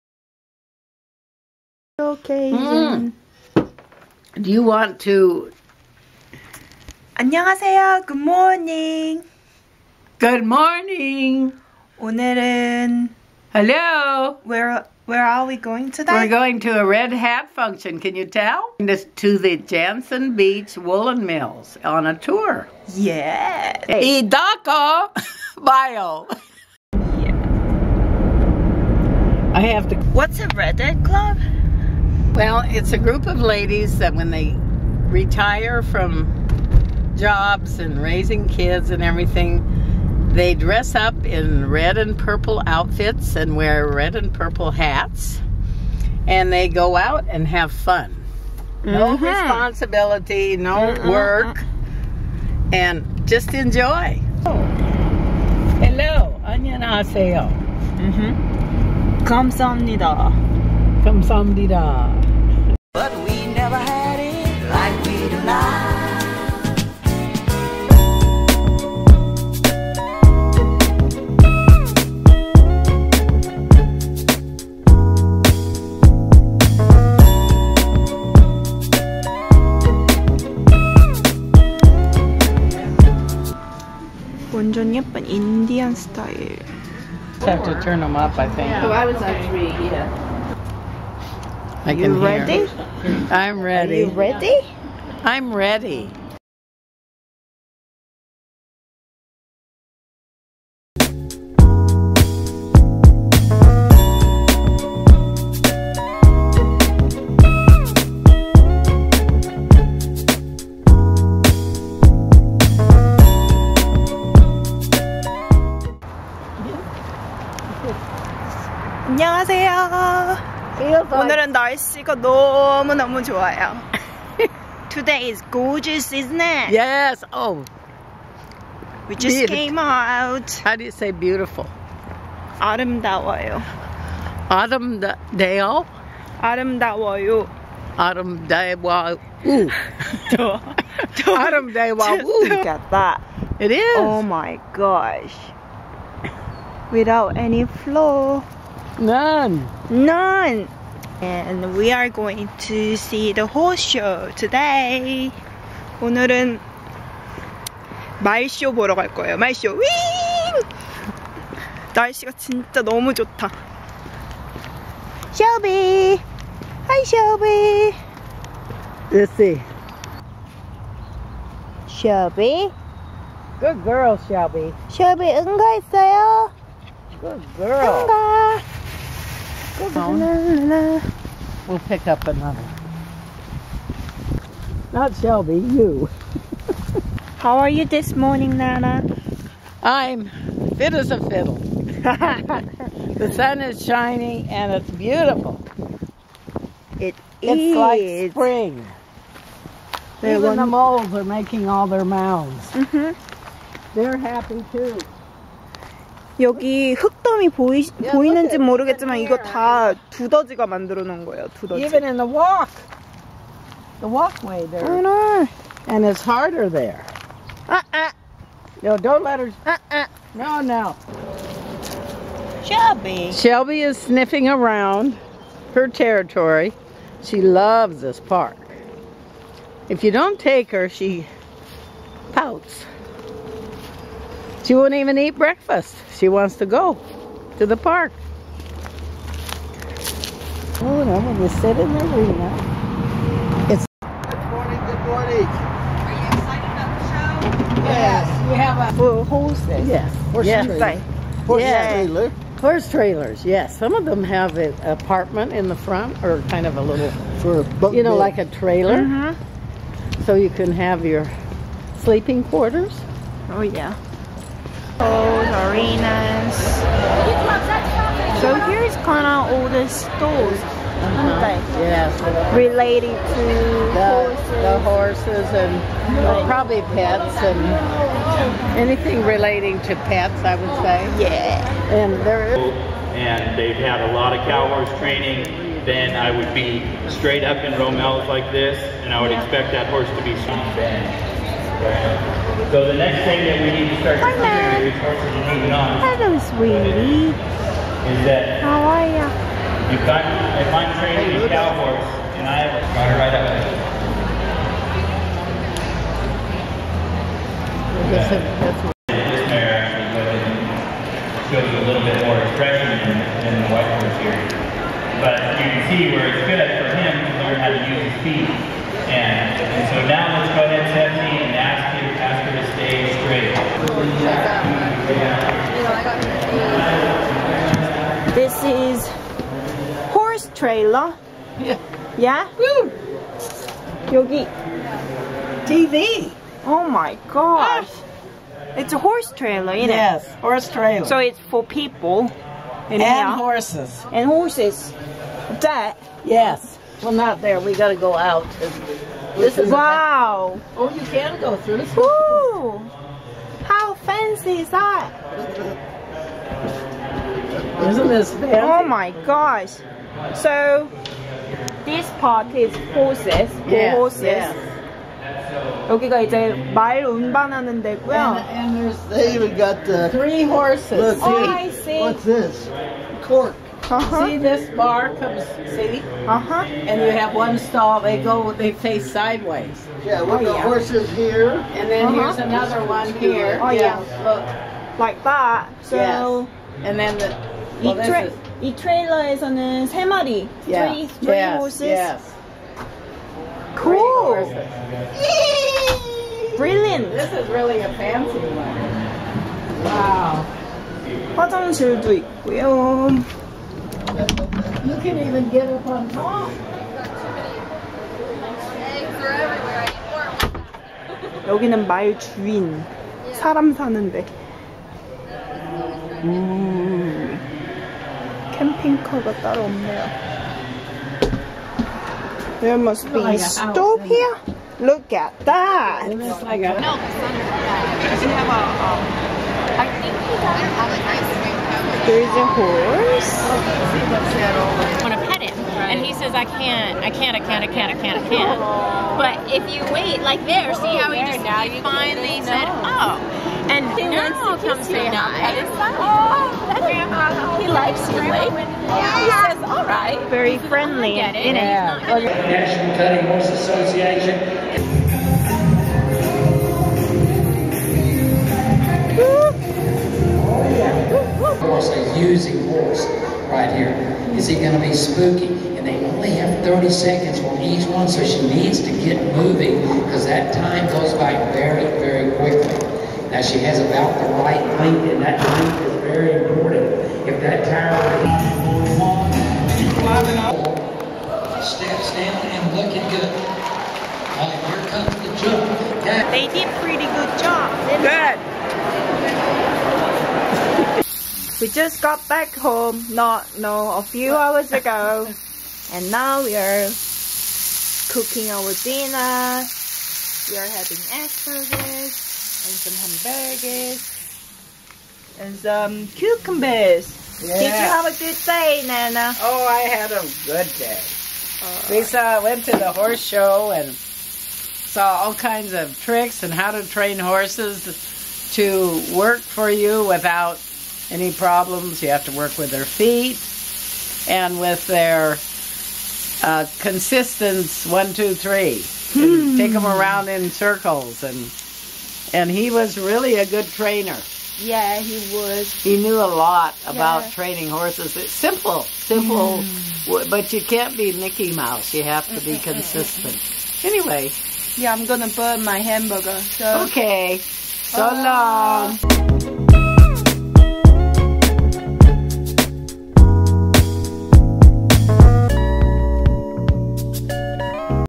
오레건 펜들턴 울공장에 구경가기 | 미국일상 브이로그 vlog 포틀랜드
This is my USA trip diary which was in 2015. My base was Vancouver WA which is a neighbor city of Portland OR.  I'm making those my life in the U.S vlogs up. Happy travels!

On that day in the clip, we went to a tour to Pendleton Woollen mills. They have such great and gorgeous pieces of clothing and blankets. It was funny cause the HQ of the company is in Oregon but the factory is actually across the river in Washington.

Also we went to see a horse show at the Clark County event center. I've been there several times, and they do host lots lots of great entertainments all year around.

▶ Infomation

＊Pendleton woollen mills Free Tour
Mon-Fri

2 Pendleton Way
Washougal, WA

17402 NE Delfel Rd, Ridgefield, WA 98642, United States